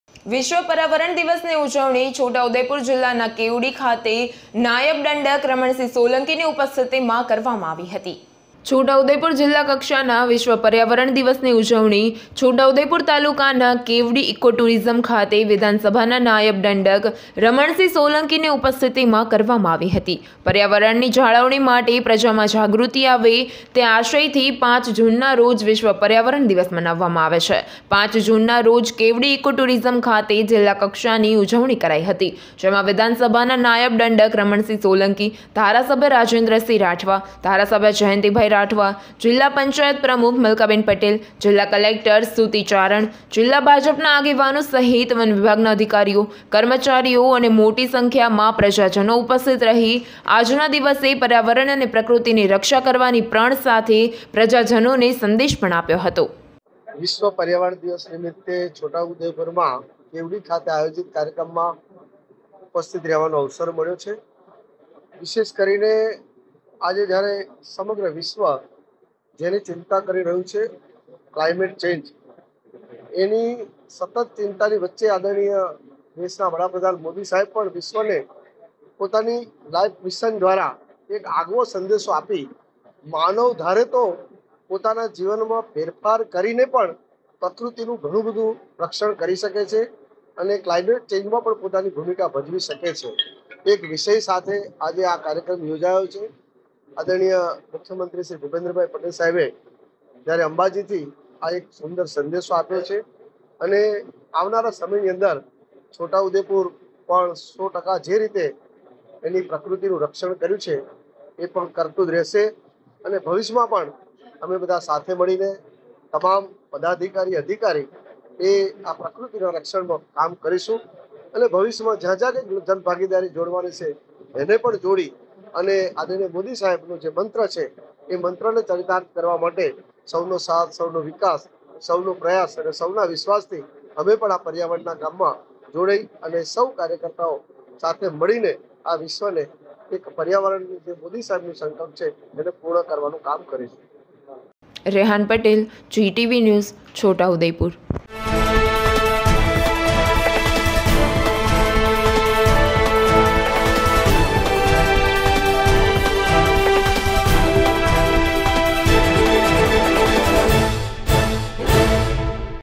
છોટાઉદેપુર:- વિધાનસભાના નાયબ દંડક રમણસિંહ સોલંકીની ઉપસ્થિતિમાં વિશ્વ પર્યાવરણ દિવસની ઉજવણી...|| GTV
છોટાઉદેપુર:- વિધાનસભાના નાયબ દંડક રમણસિંહ સોલંકીની ઉપસ્થિતિમાં વિશ્વ પર્યાવરણ દિવસની ઉજવણી...|| GTV_GUJARAT_NEWS